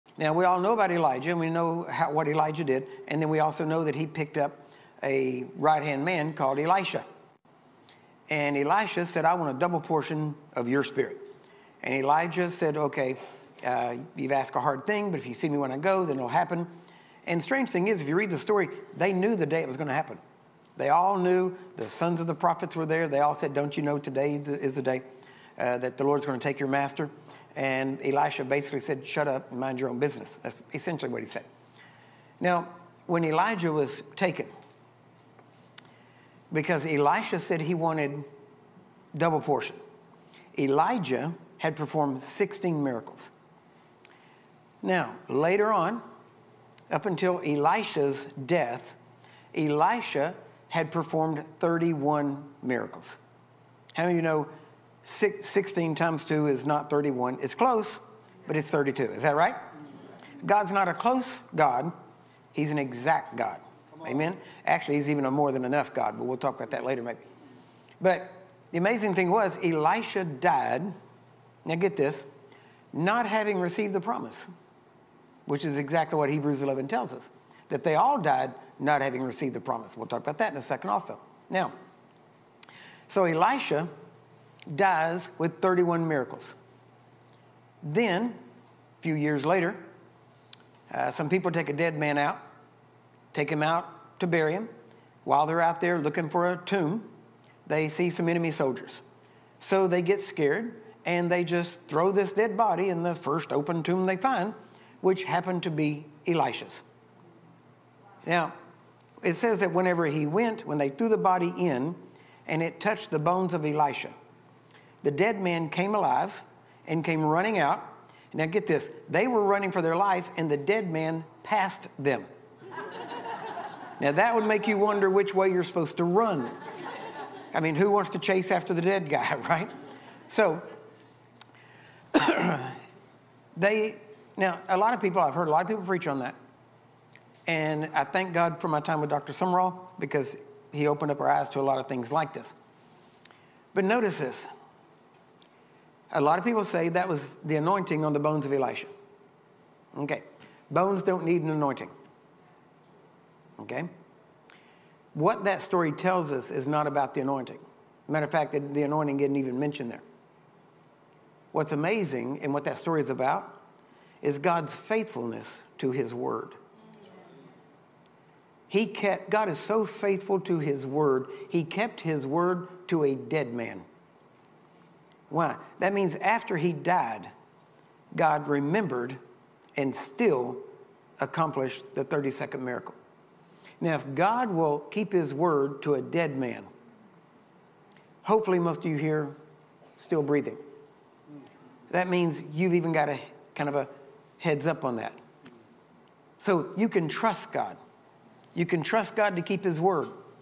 Elisha: God's Faithfulness to His Promise, Curry Blake
In this video, Brother Curry Blake shares a trivia about Elisha's life and uses this anecdote to demonstrate God's faithfulness to his word.